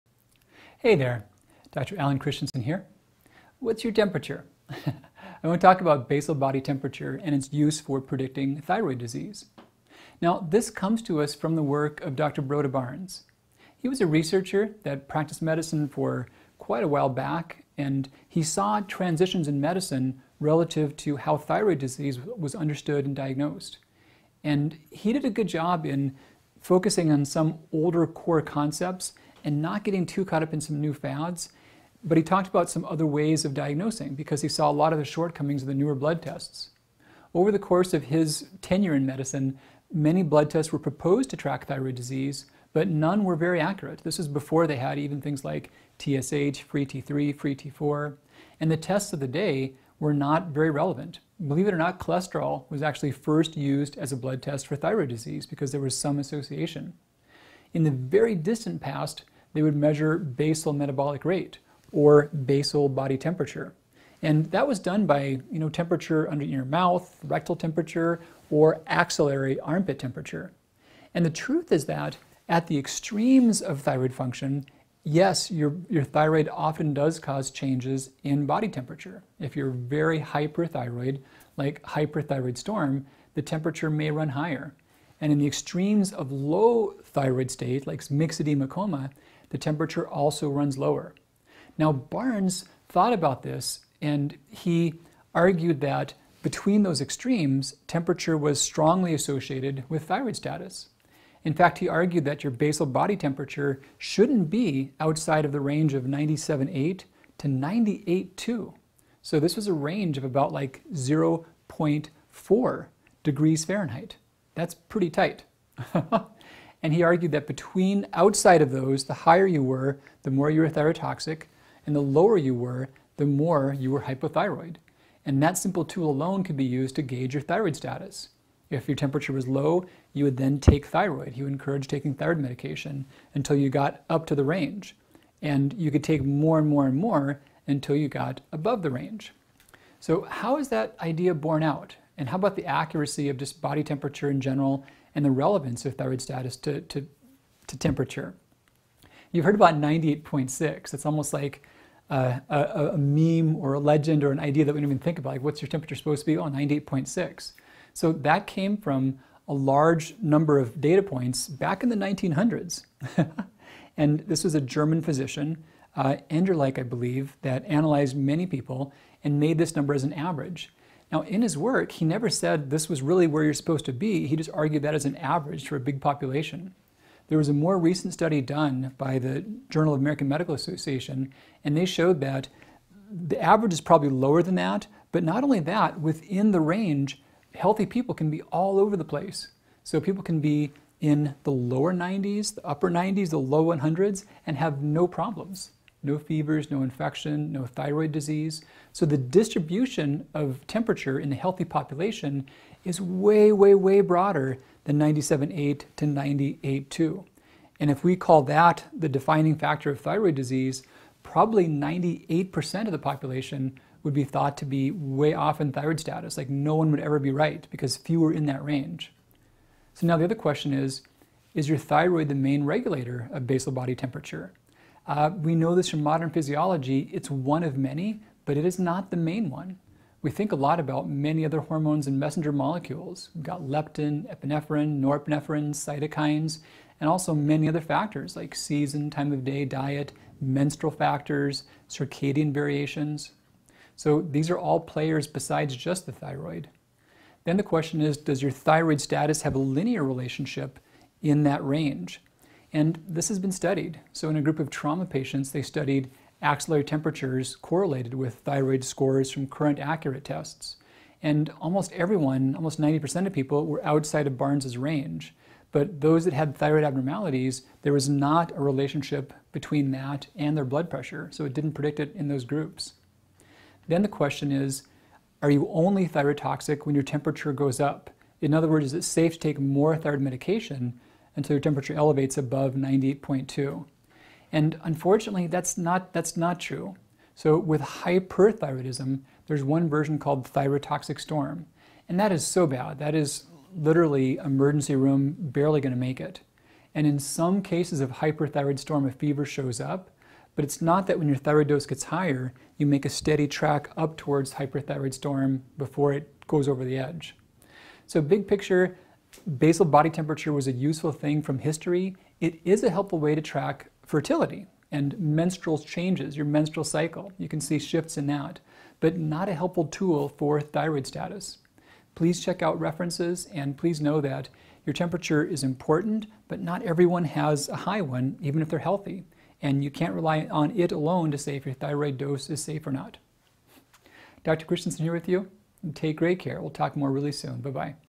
Comprehensive guide to basal body temperature for thyroid
Do you know how to test your basal body temperature to see if your thyroid is working? Learn the full story behind temperature testing.